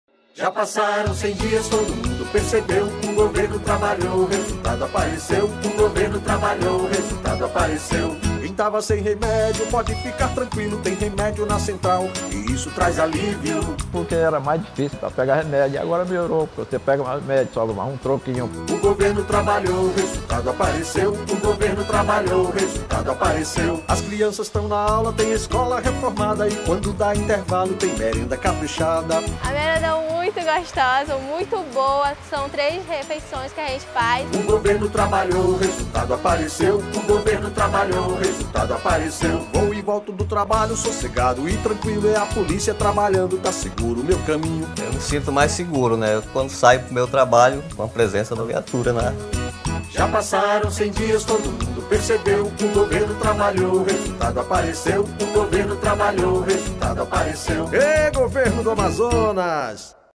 100 dias: Em vídeo, Governo do Amazonas destaca ações na saúde, educação e segurança
Em vídeo publicitário divulgado nesta segunda à imprensa, o governo destaca ações como compra de medicamentos, reformas de escolas e policiamento nas ruas.